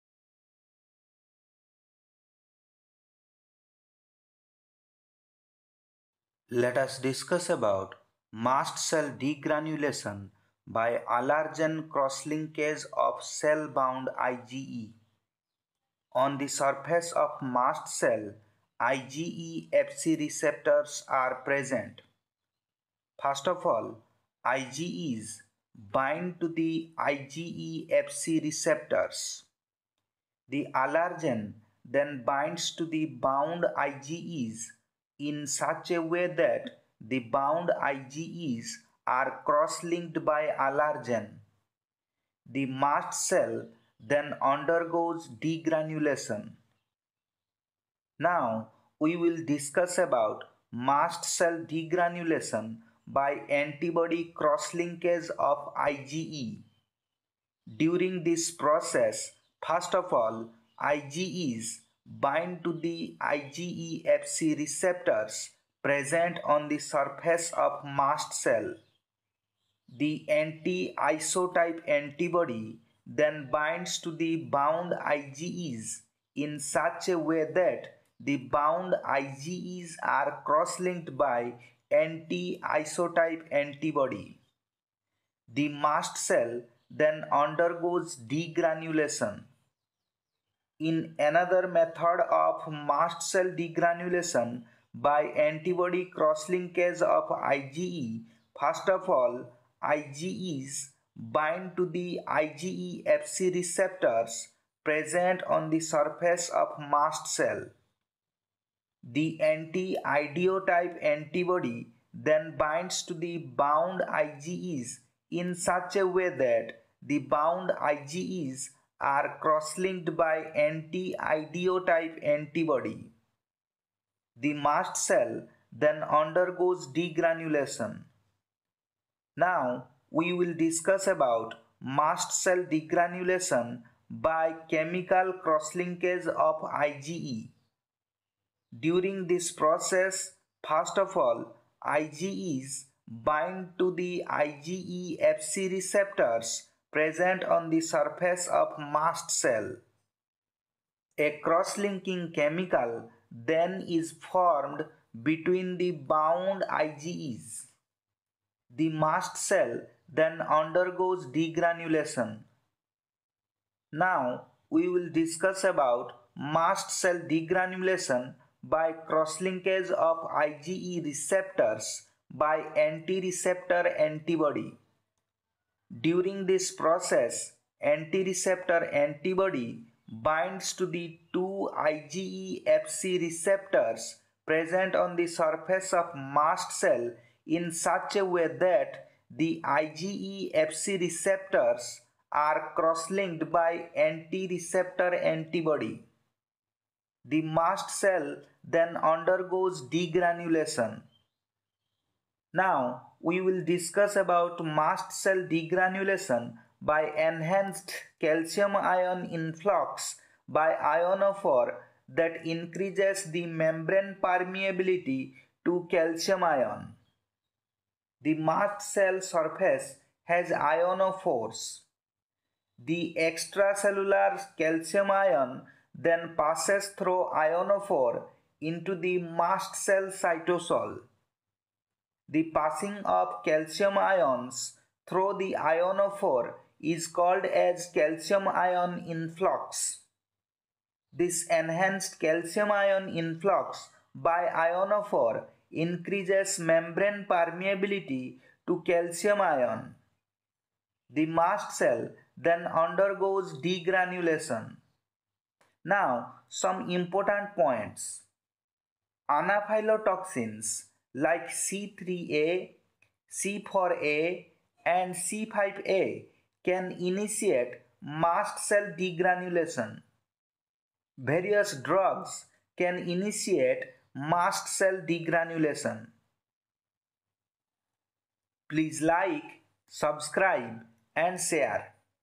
Mast cell degranulation mechanisms
In this video we have discussed about mechanisms that can trigger degranulation of mast cells .